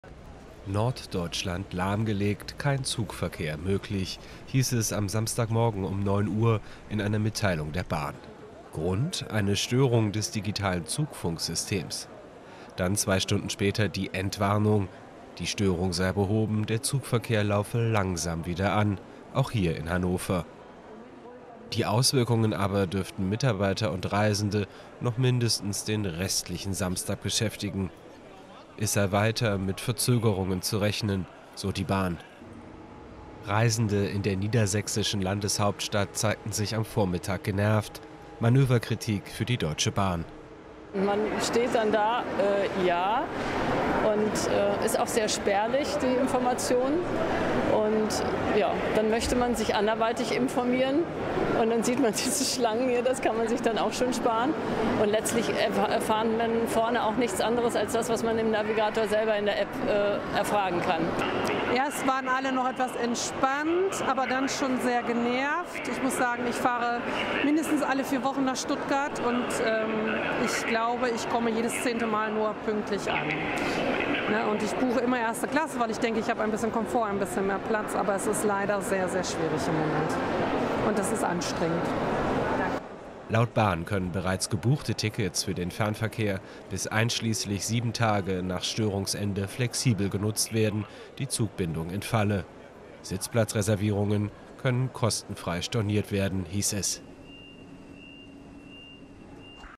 Deutsche Bahn im Norden komplett lahmgelegt: Störung ist behoben, Verspätungen dauern an
Eine Störung im digitalen Zugfunkverkehr hat die Deutsche Bahn am Samstagmorgen im Norden Deutschlands zum Stillstand gebracht. Auch wenn das Problem mittlerweile behoben ist, die Reisenden werden voraussichtlich noch länger mit Verspätungen rechnen müssen.